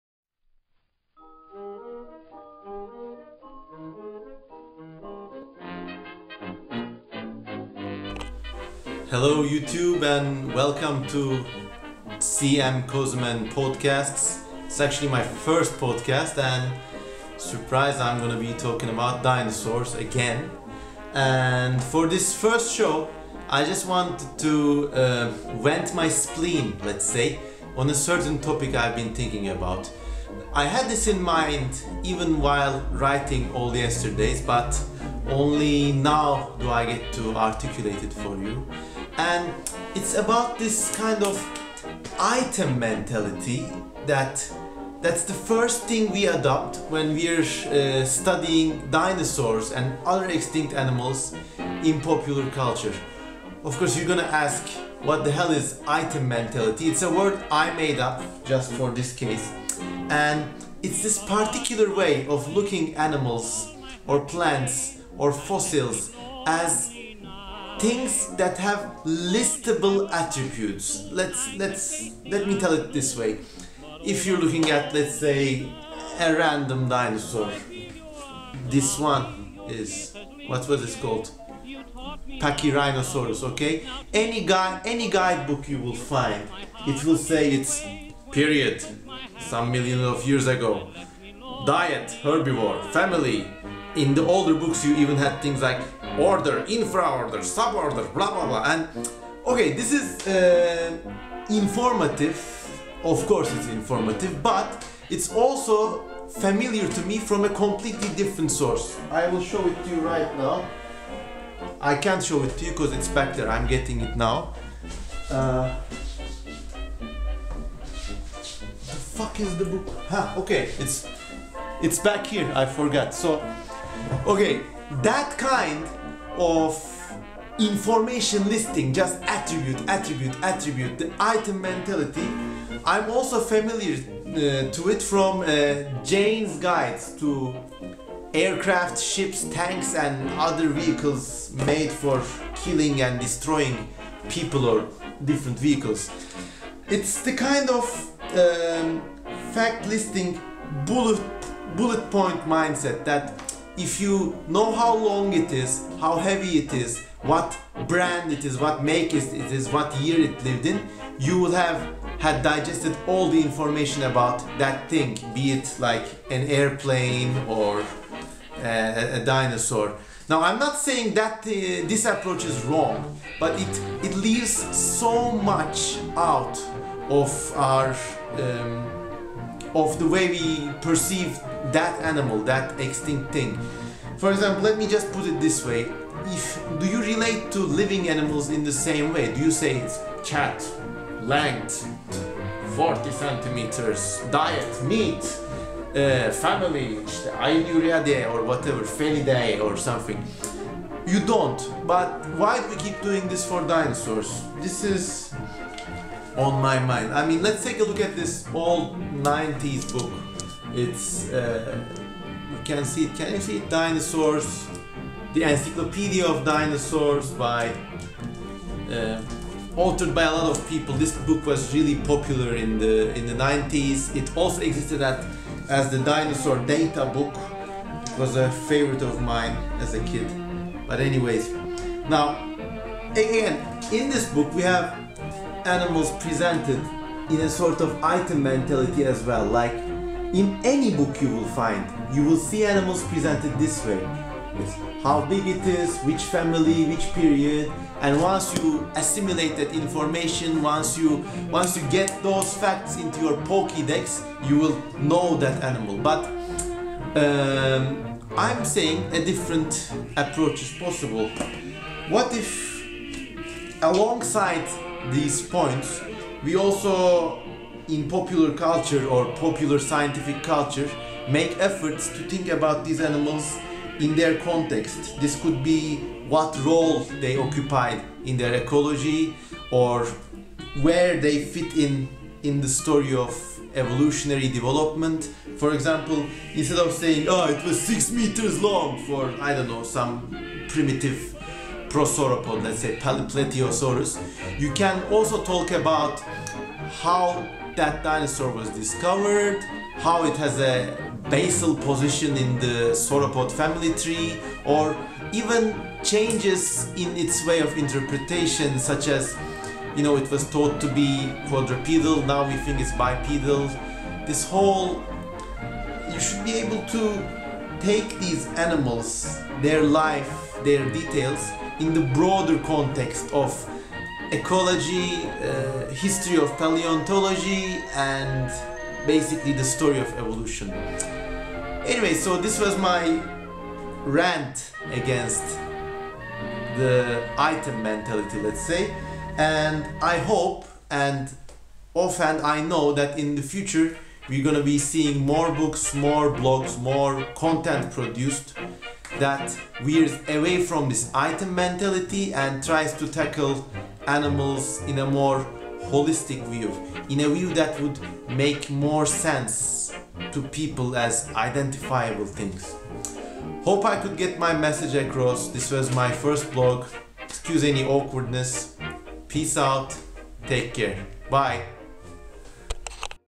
"Item Mentality" and Dinosaurs in Popular Science
My first video podcast - a rant against the "item mentality" so common in books about dinosaurs. Why is information about extinct animals always boiled down to a list of figures; length, diet, time period, family, etc. and so on. These facts aren't useless in themselves, but concentrating exclusively on them prevents us from looking at the past in a broader perspective.